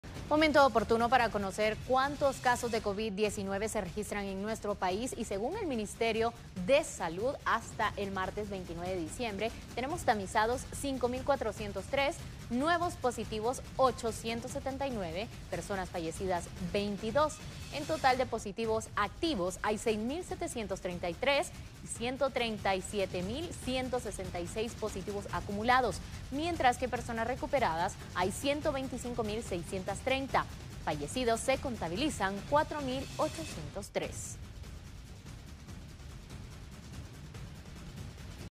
Revisamos número de casos Covid-19 en Guatemala
De acuerdo a las cifras publicadas por el Ministerio de Salud Pública y Asistencia Social, Guatemala mantiene 6 mil 733 casos activos de contagiados de Covid-19.